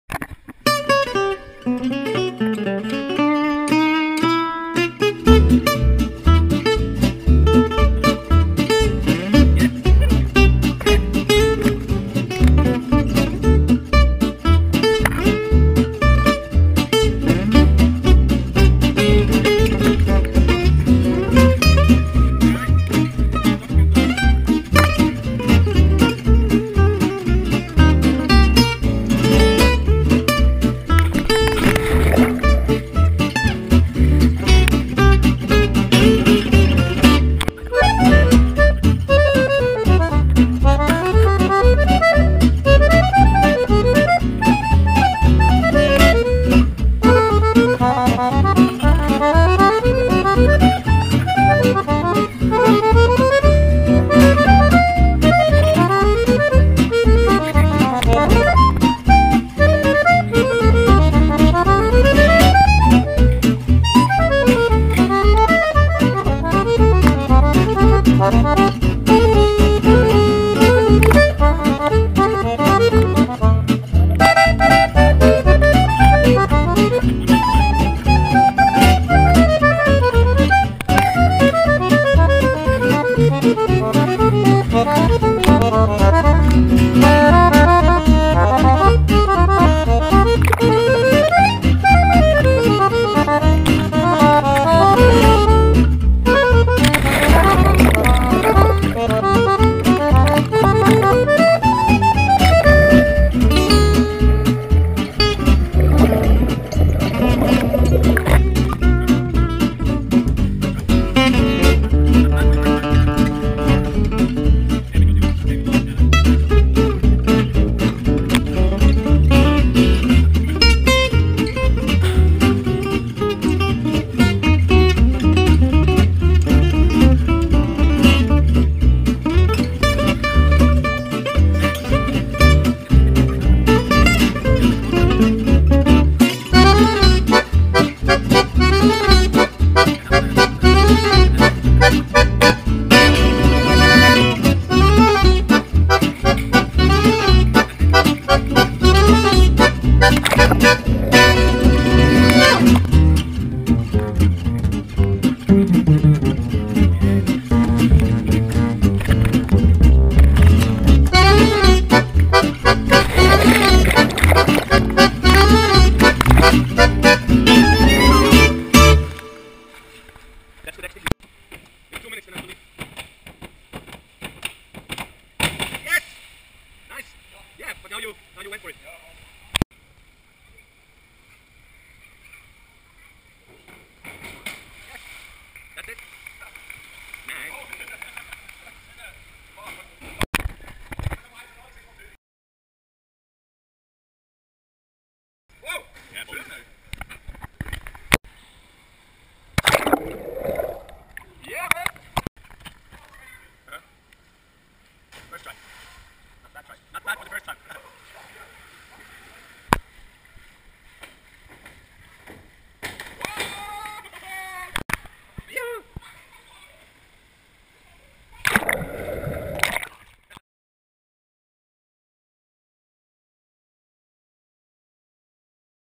Diving Board Tricks FAILS GoPro - Slapstick-Style Fail Video - Diving Board
Diving Board Tricks FAILS - Slapstick-Style Fail Video - Diving Board
Double Frontflip - Flip Fails - 42 jumps
Preparing for Double Backflip - we meet up every week now in winter to practice new tricks like all de double flips and tsukahara. in spring we should then be ready to take it into the canyons. we love the adrenaline and failing/get hurt together is so much more fun :-)
Learning double backflips into water hurts much less than on the ground itself. Canyoning Season 2017 will be taken to the next level.
this is more of a fun video, slapstick-style cut :-) soon there comes a video with us actually sticking the tricks :-)